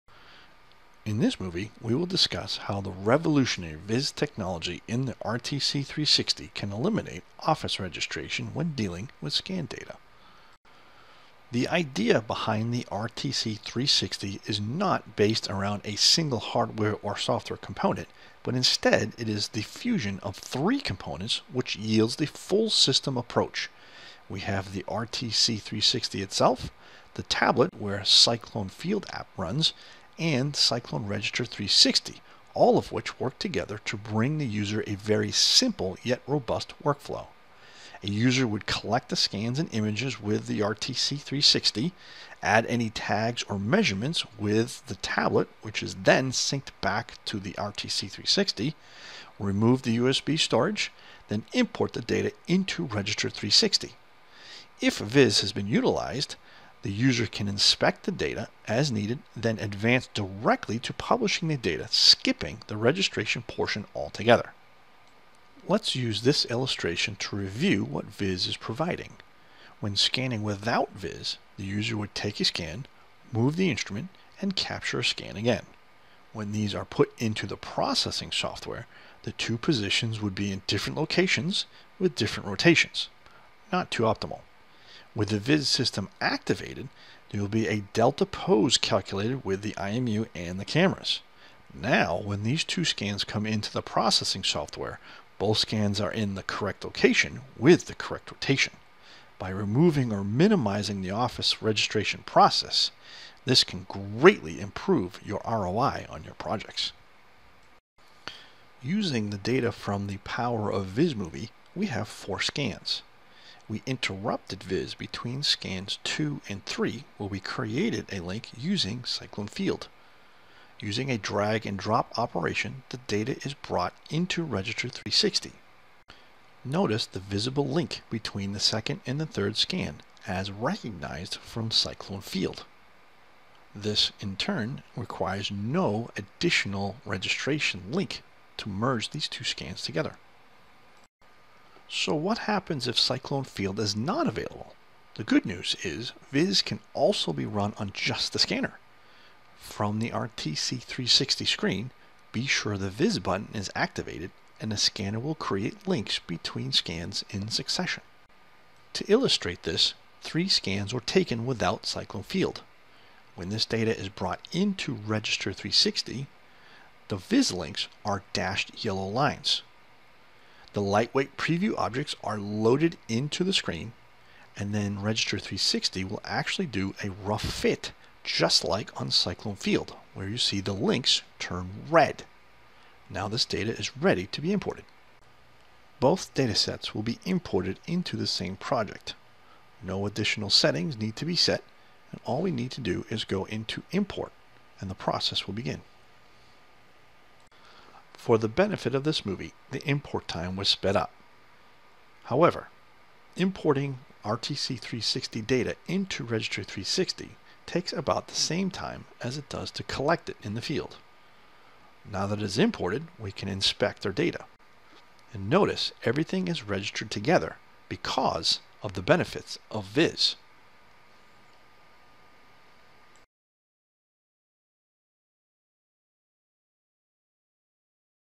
RTC360: Eliminating Registration with VIS
The Leica Geosystems RTC360 uses VIS technology and it SEES EVERYTHING! Please spend some time and learn more on the power of VIS and how you can all but ELIMINATE your registration workflow!

The scanner is aware of its position in real-time it senses its position from one scan to the next.

(Video Credits to "Harvey Productions", Mike Harvey, Reality Capture Guru with Leica Geosystems)